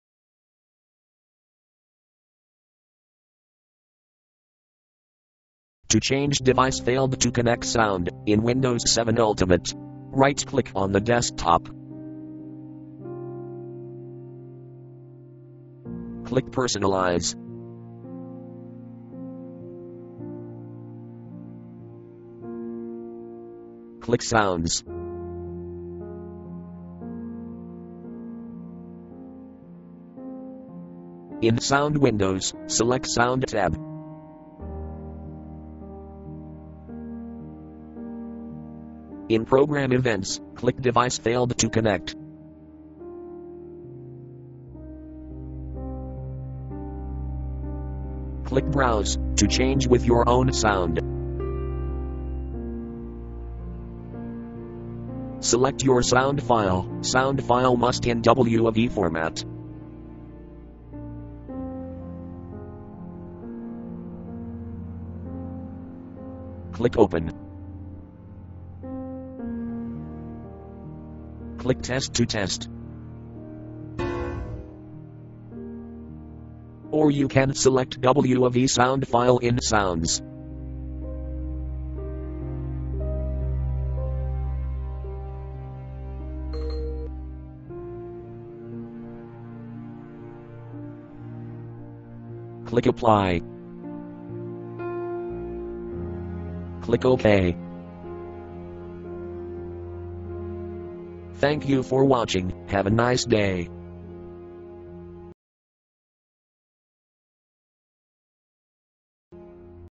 Windows 7 Tips : How to Change Device Failed to Connect Sound (Program Events)
This video show How to Change Device Failed to Connect Sound in Windows 7 Ultimate